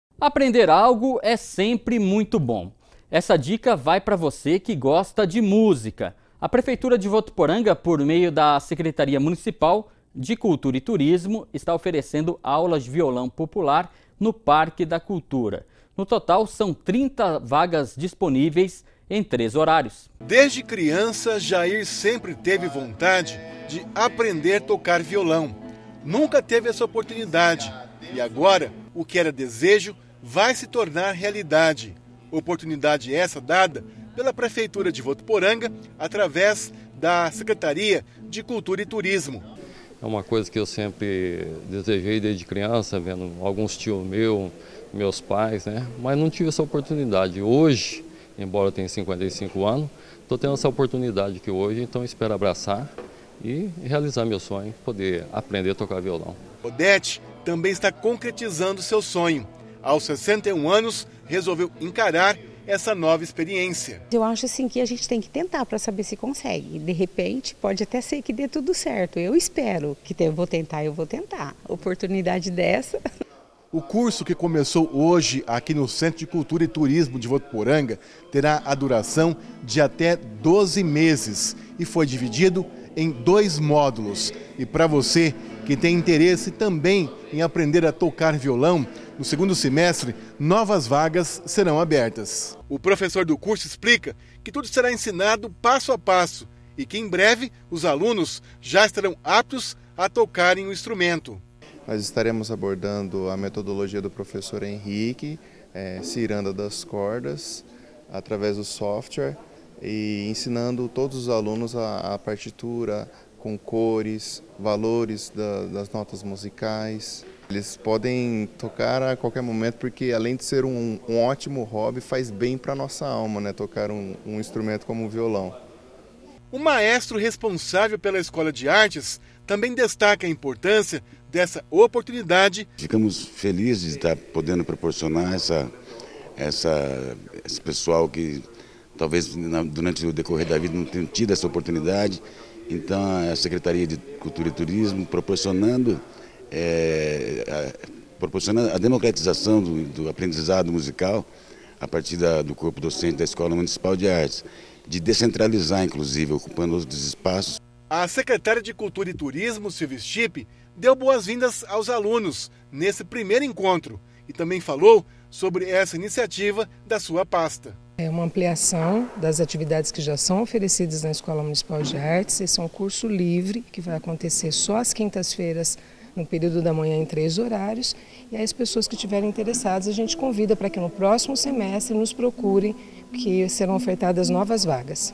Aulas de violão popular no Parque da Cultura
Essa dica vai para você que gosta de música. A Prefeitura de Votuporanga, por meio da Secretaria Municipal de Cultura e Turismo está oferecendo aulas de violão popular no Parque da Cultura. Confira.